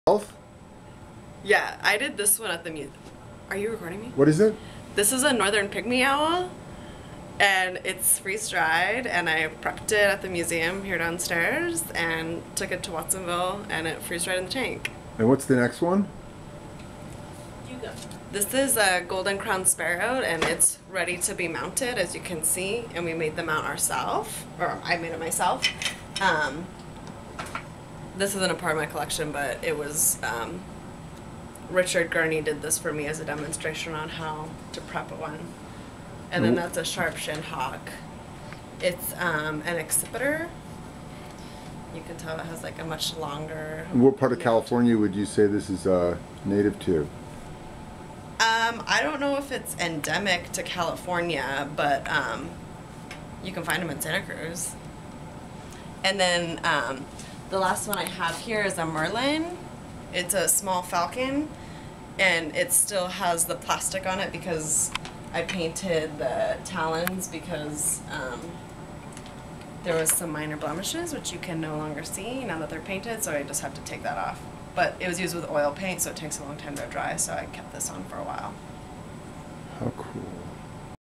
Environmental Studies Museum at UC Santa Cruz
ENVS MUSEUM--The Museum of Natural History Collection (MNHC) has a dedicated and lively group of volunteers and student interns who engage with each other and the MNHC curator, sharing their knowledge and passion for natural history and museum techniques.  Student interns and community volunteers have the freedom to engage in projects of interest to themselves, developing their knowledge of specific organisms or museum management skills.   Undergraduate interns may either choose a 2-unit or 5-unit internship sponsored through the Environmental Studies internship office.